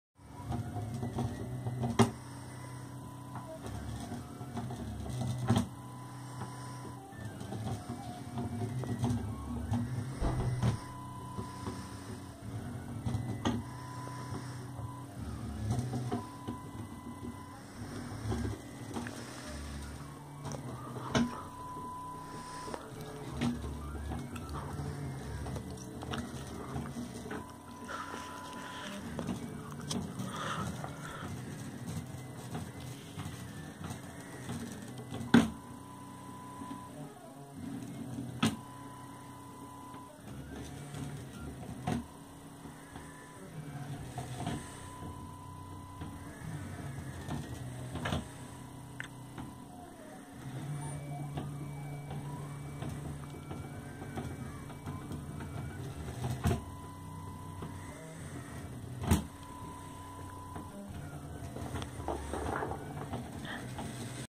robocup rescue
Training for Brazil's robocup rescue for Fundação Liberato.